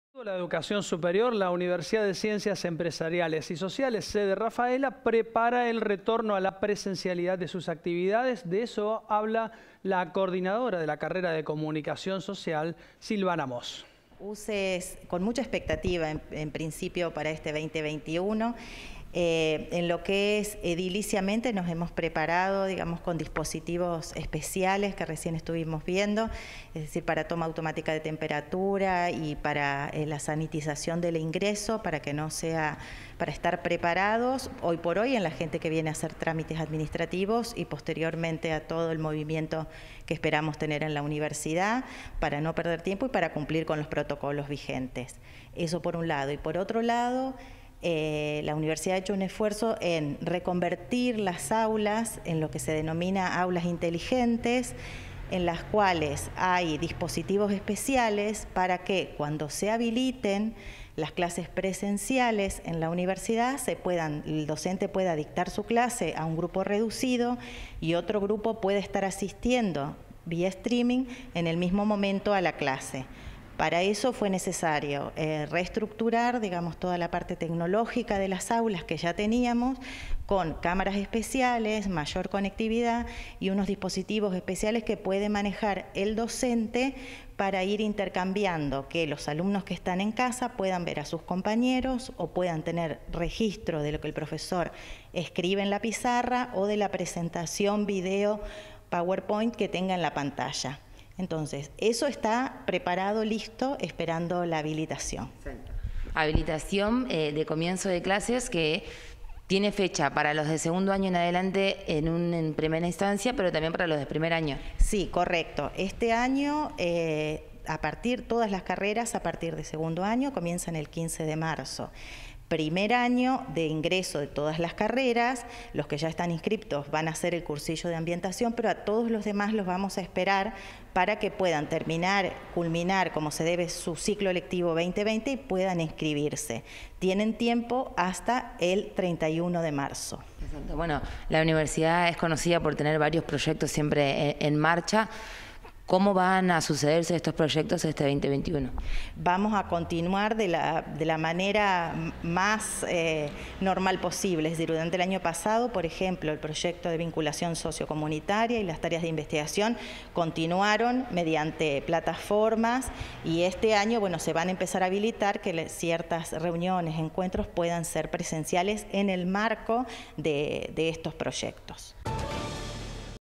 UCES PREPARA EL REGRESO A LA PRESENCIALIDAD
SILVANA MOOS - COORDINADORA LIC. EN COMUNICACIÓN SOCIAL